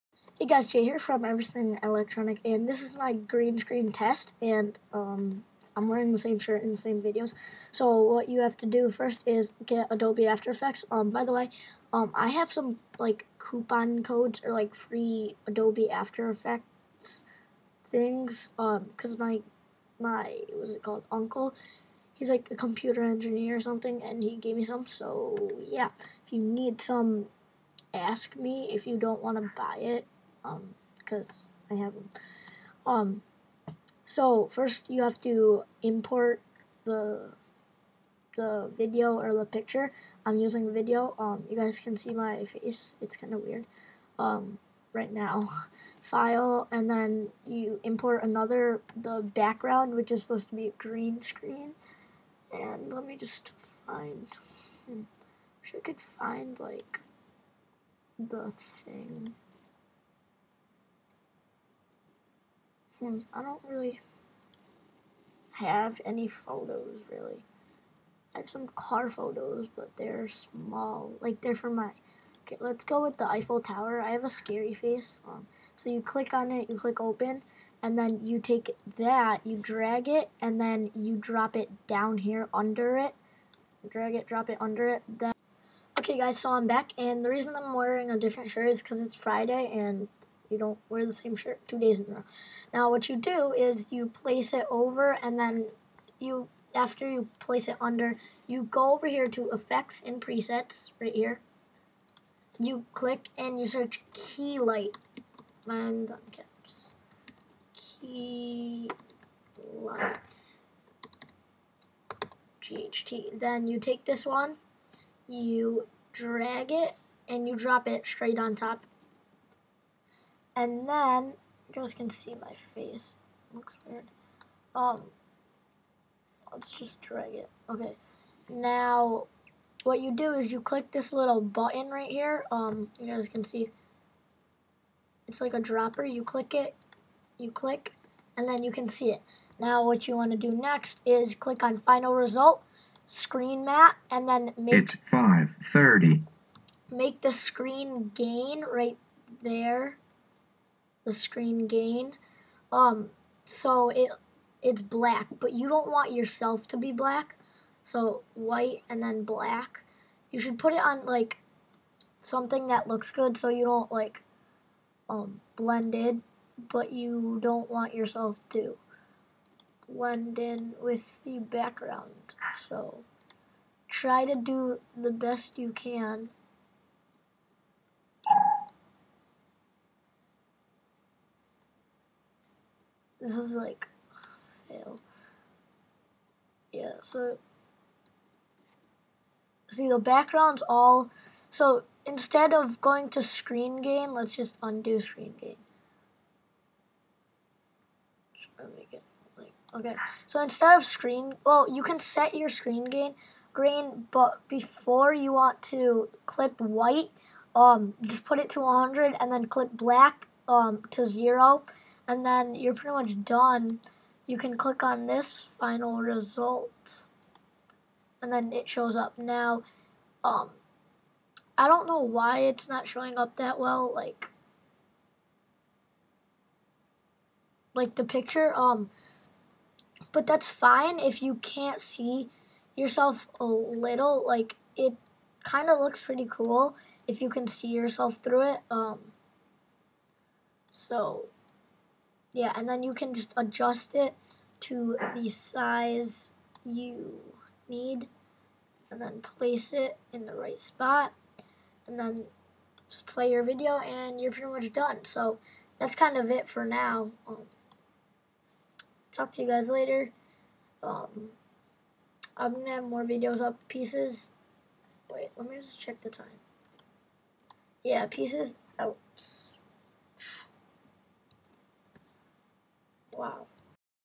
How to Use a Green Screen-Adobe After Effects
He guys in this video I show you how to use a green screen. Now the final result wont be amazing especially if you have a video. Pictures look a lot better but it doesnt really matter. Now while doing this I found out you have to have an even amount of lighting in every place. To much lighting will throw everythin off. Tip- Dont wear green or blue.

Schedule: Every other day. If I miss a day I will make it up the next day.

AIM: everyelectronicY

Skype: Ask me for it.